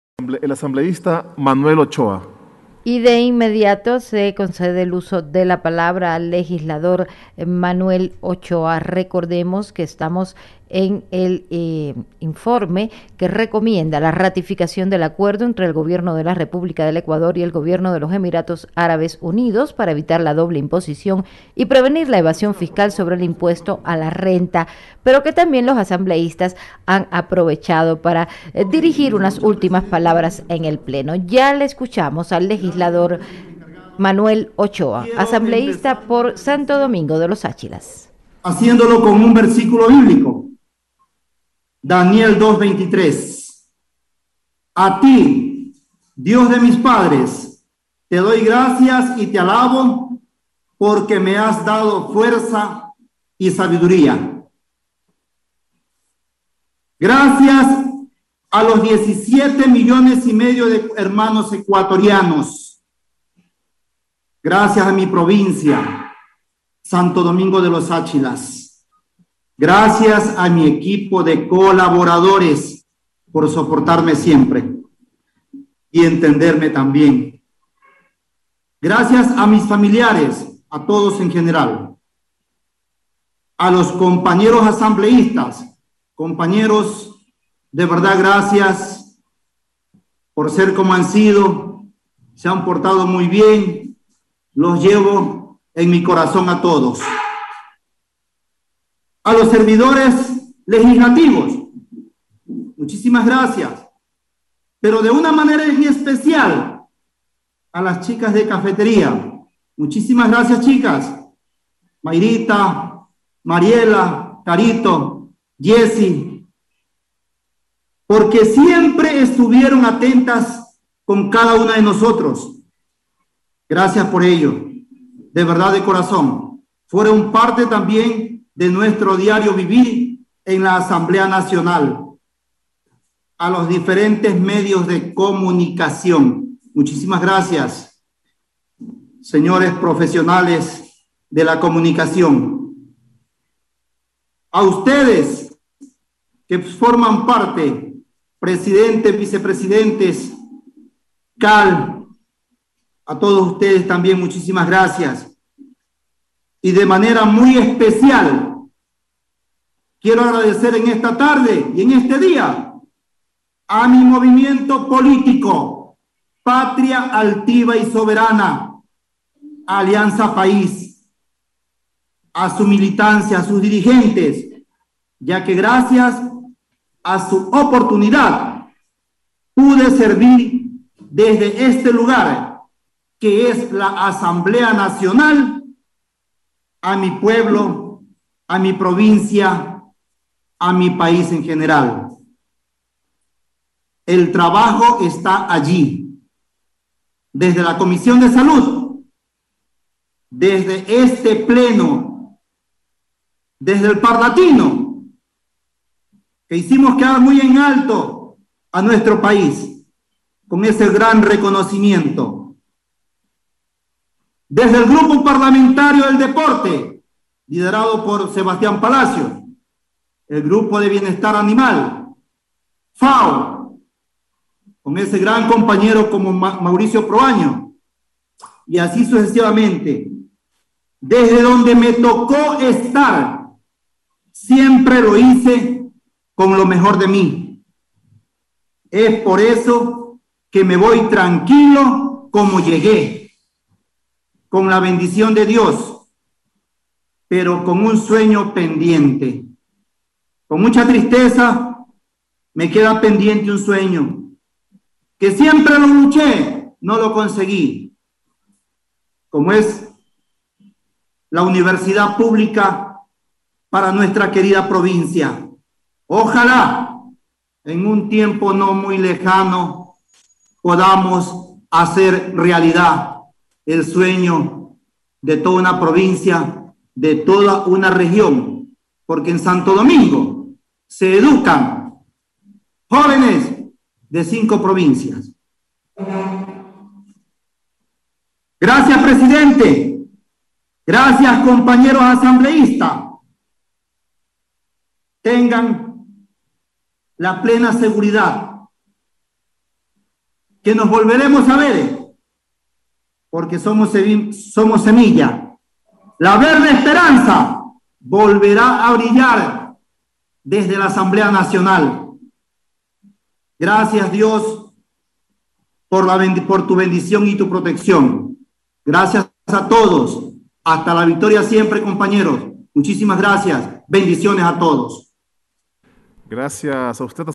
Manuel Ochoa - Sesión 704
11 de mayo de 2021
Informe que recomienda la ratificación del Acuerdo entre el Gobierno de la República del Ecuador y el Gobierno de los Emiratos Árabes Unidos para evitar la doble imposición y prevenir la evasión fiscal sobre el impuesto a la renta.